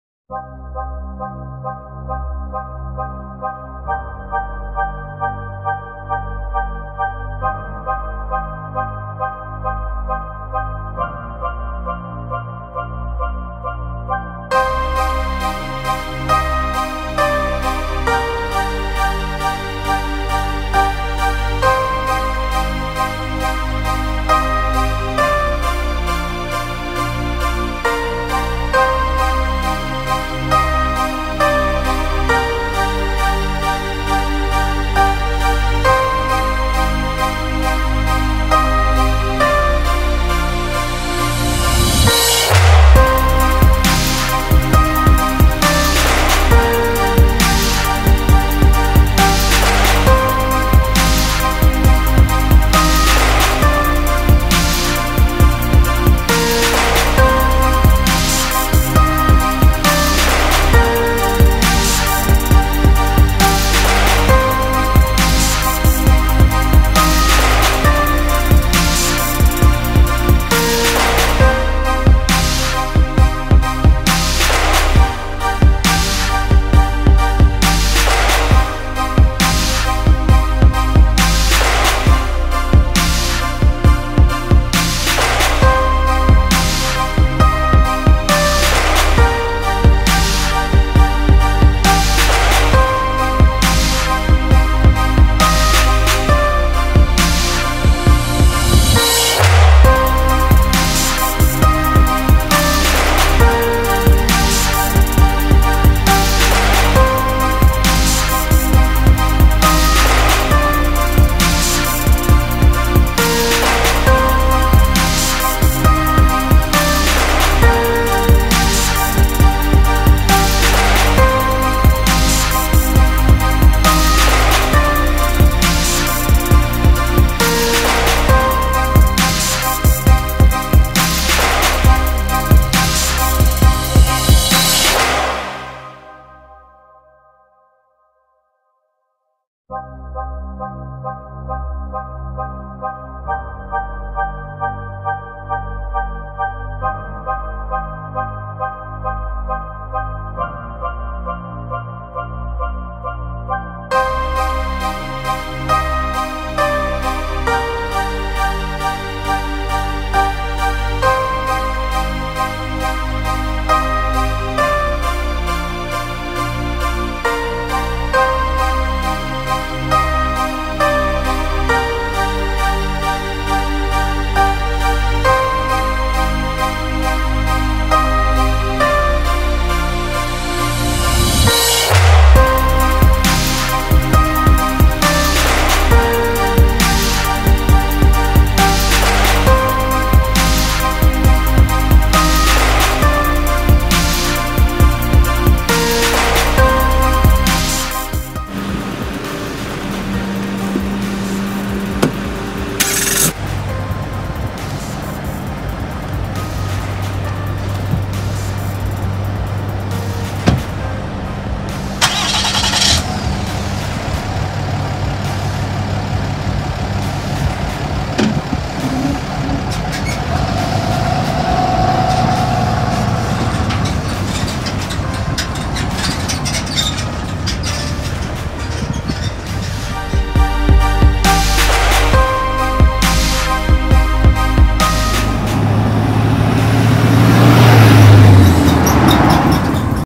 Car hauling made easy.😏 (Part 1)
A quick video of three 2016 cruzes getting loaded in Indiana and then heading to a dealership in Wichita Kansas.. (Part 1)